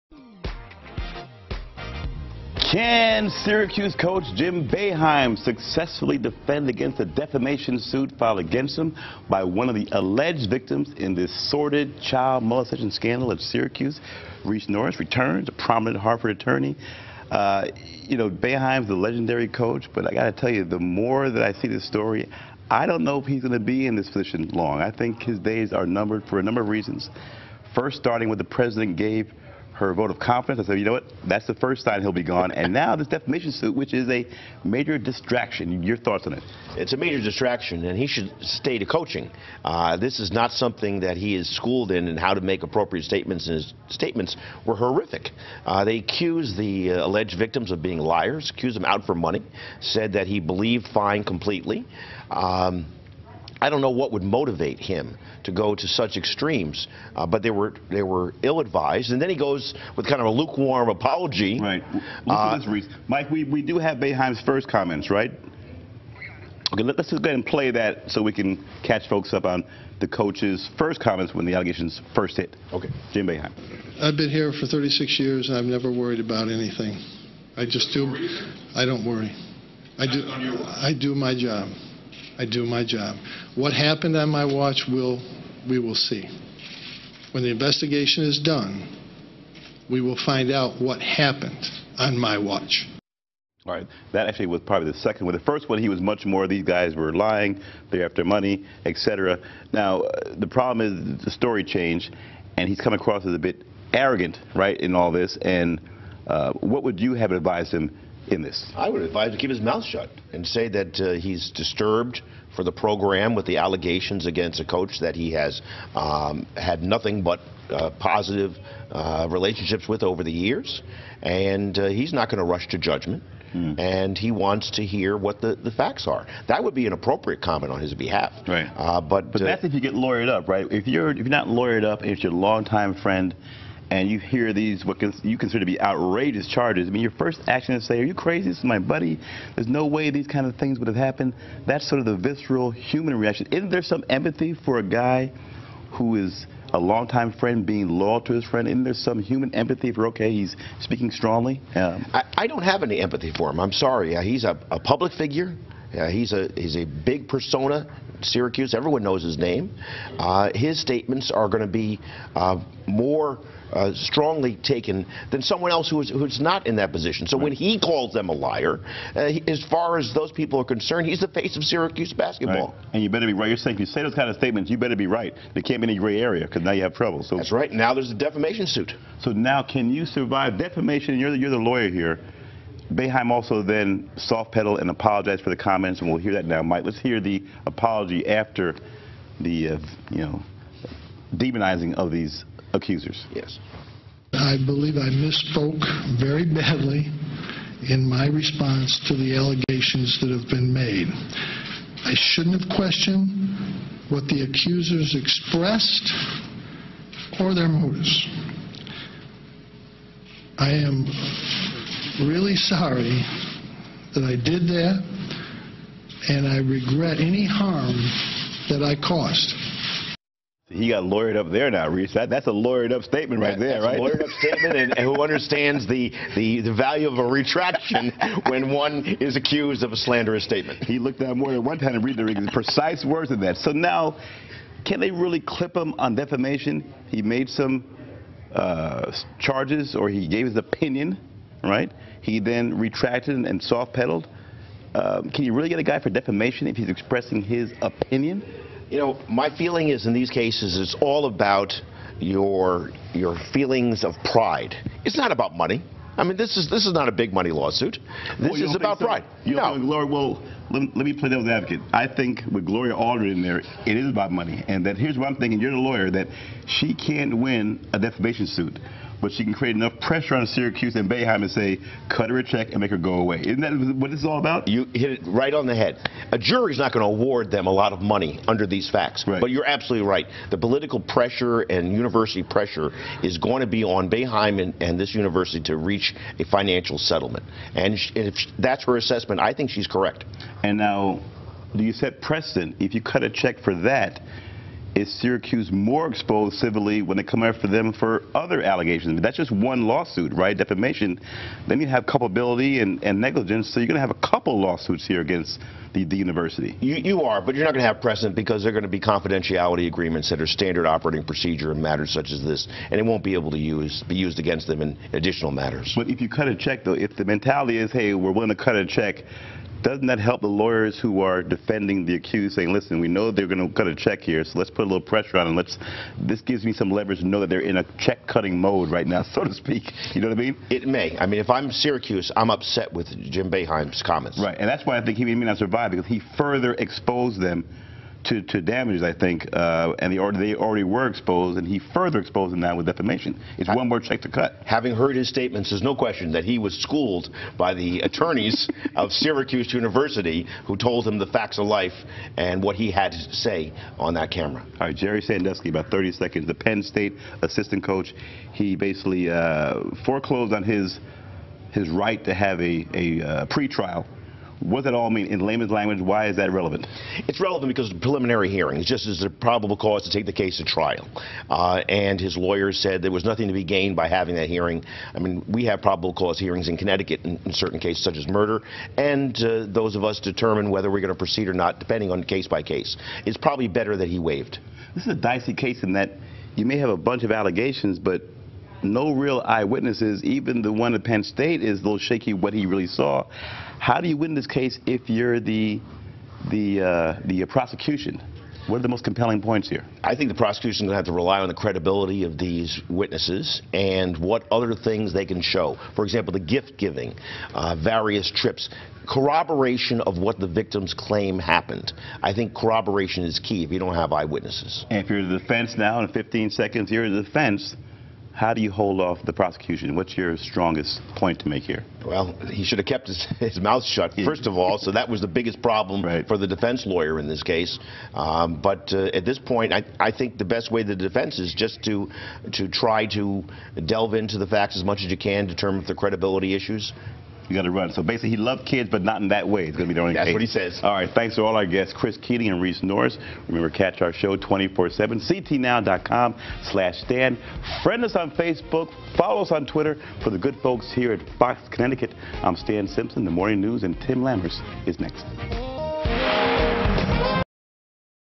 Stan Simpson: Penn State & Syracuse Scandals | 12/18
Reese Norris, defense attorney, talks about the continuing child molestation scandels by former coaches at Penn State and Syracuse University.